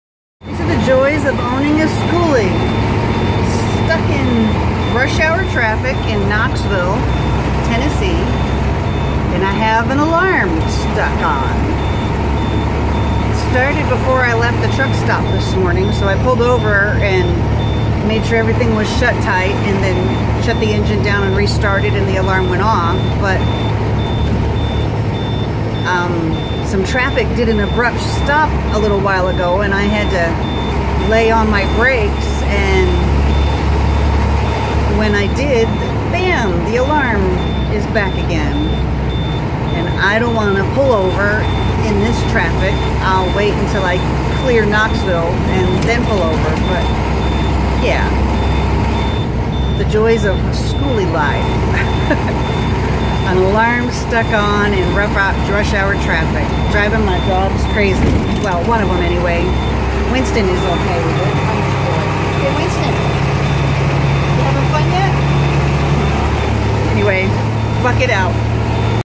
Rush hour Knoxville and the dreaded alarm
Oh the joys of driving a skoolie!  The dreaded unknown alarm at the most inconvenient times...this one was because some stuff in the back shifted and the back door alarm wasn't making contact